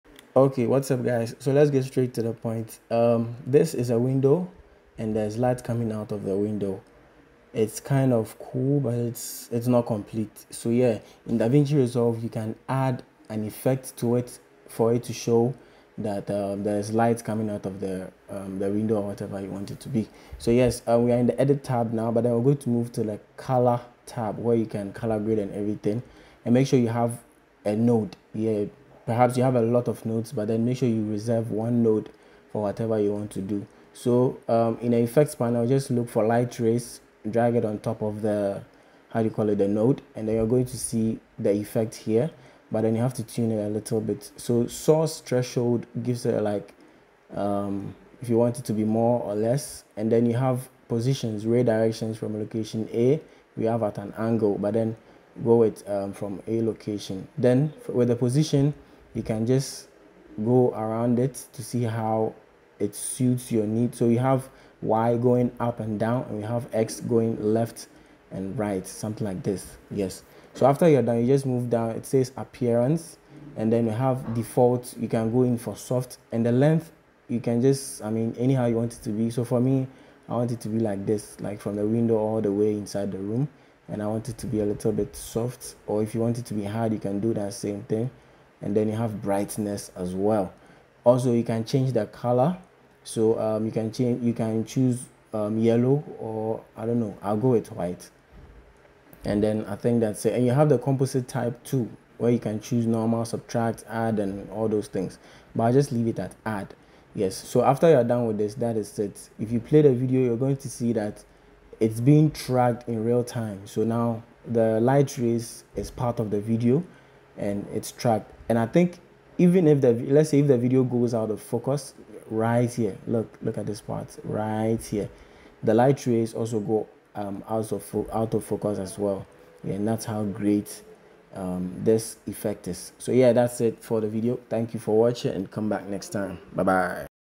The EASIEST WAY to create FAKE LIGHT RAYS in DaVinci Resolve
I'm a Graphic Designer, Content Creator, Creative Director, Photographer, Videographer from Ghana. I'm currently into Content Creation, Videography and YouTube.